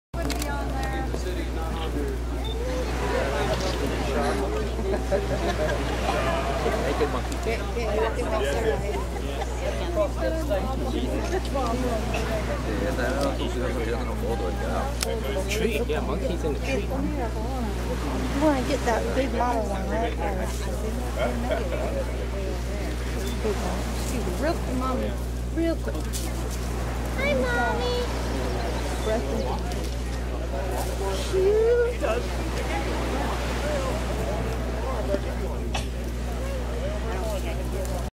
Monkeys in St Kitts at Shipwreck Grill & Bar
Monkeys roam around at the grounds of the Shipwreck Grill & Bar in St Kitts at the beach.  See more photographs of the beautiful island of St Kitts, plus the monkeys and the beach scene here: